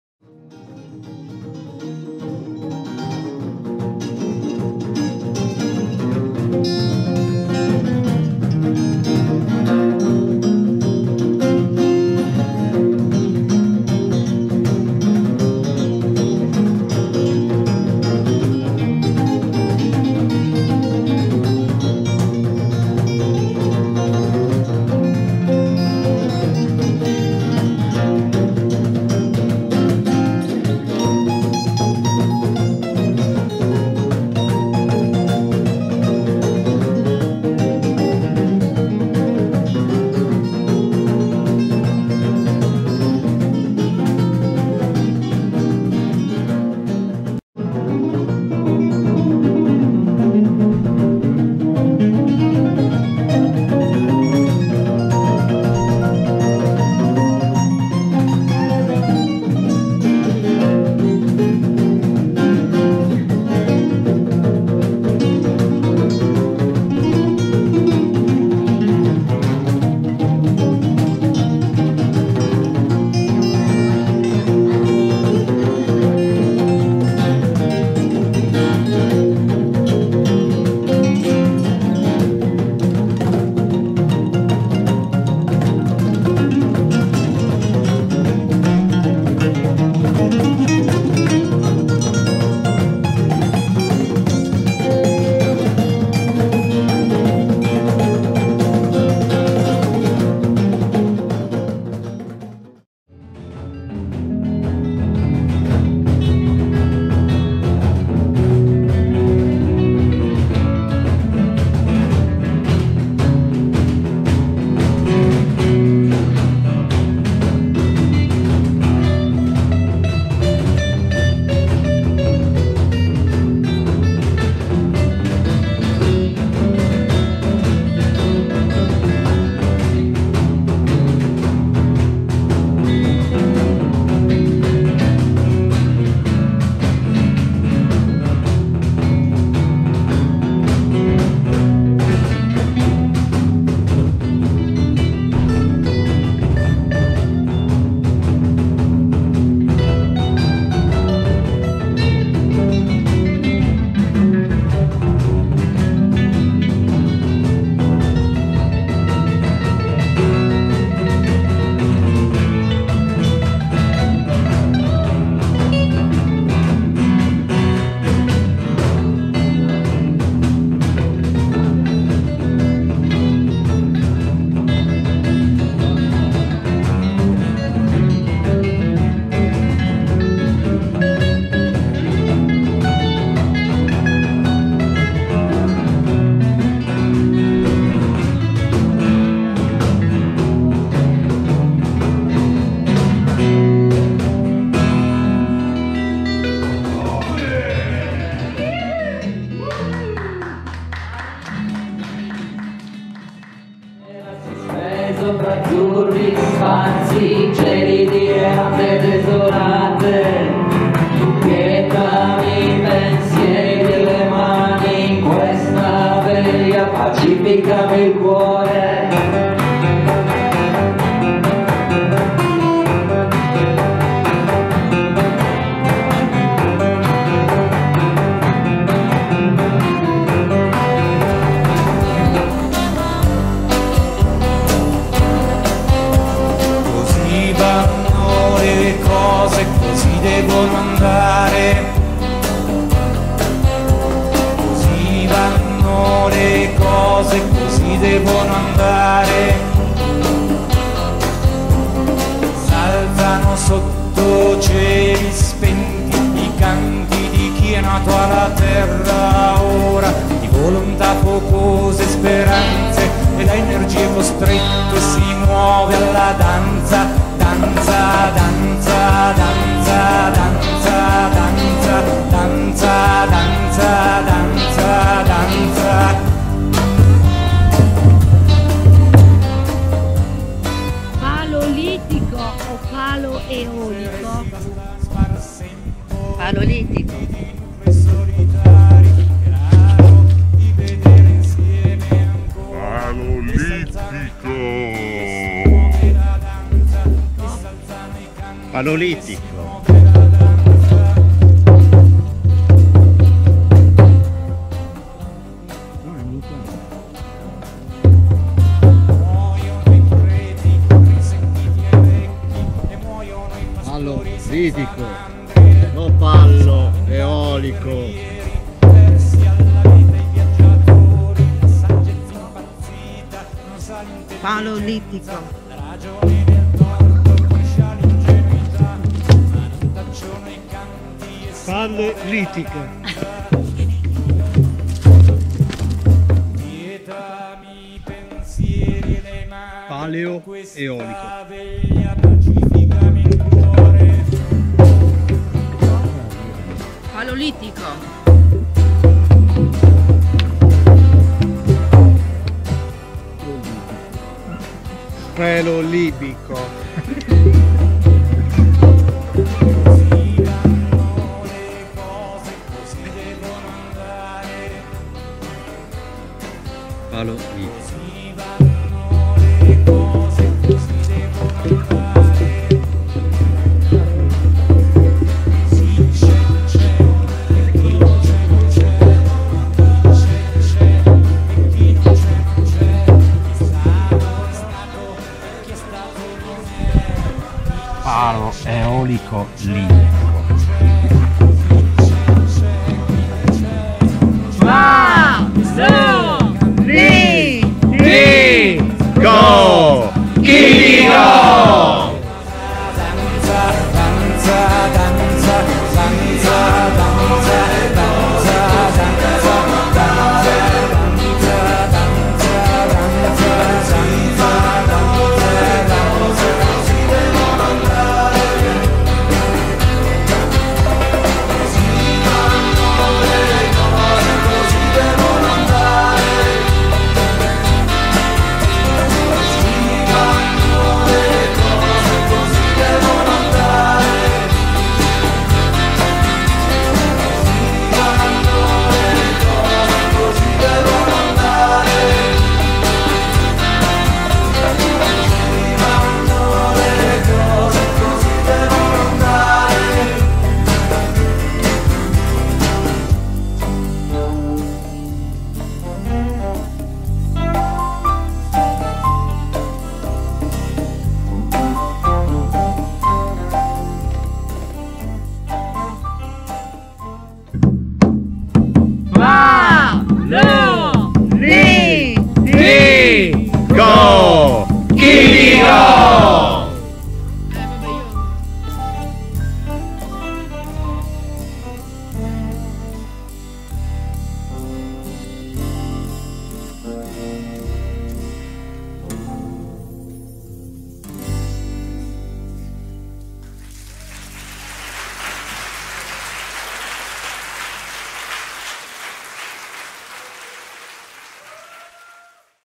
Dal Pal(e)o Litico al palo eolico
Azione artistica nel territorio e per il territorio, contro ogni forma di speculazione.

Una giornata con amici per manifestare il nostro SI al territorio. Così, come l'abbiamo conosciuto, ed è così che vorremo lasciarlo ai figli dei nostri figli, da cui lo abbiamo avuto solo in prestito. (Prov. Sioux)

Grazie a tutti i partecipanti:
Musicisti: Kasia, Igor, Giacomo, Ivano, Antonio, Simona.
fotografie e video: Roberto e Ivano.

A cura dell'associazione Boghes